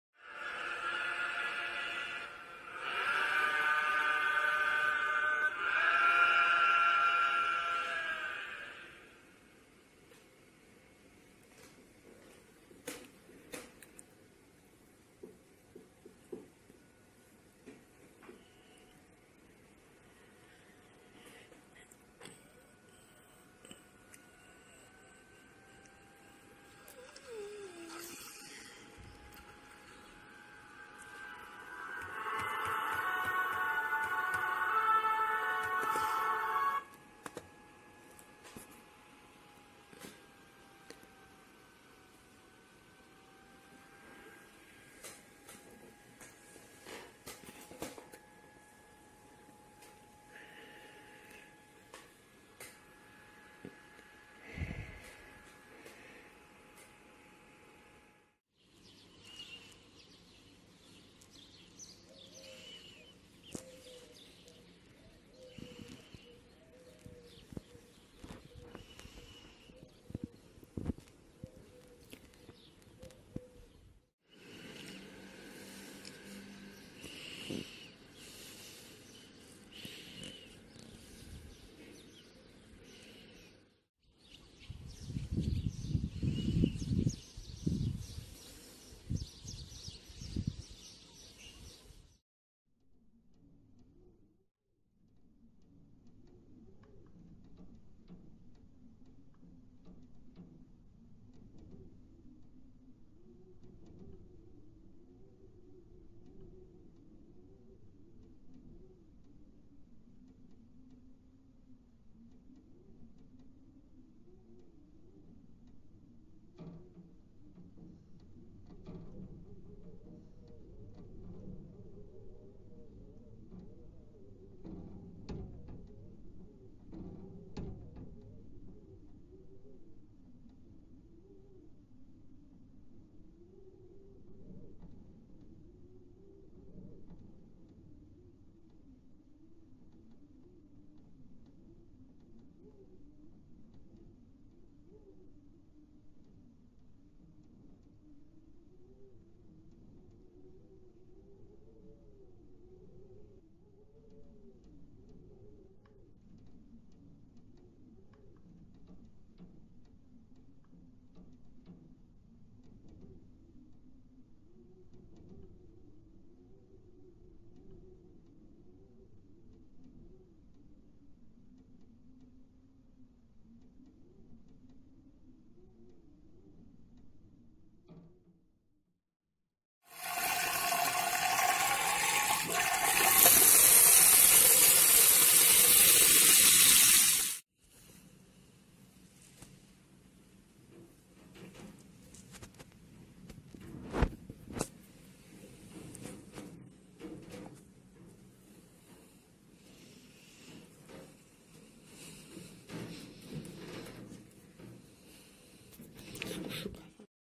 Le Sere ai Tempi del Coronavirus 🖤
Vi racconto con questo video porno di suoni, le mie sere in questo periodo buio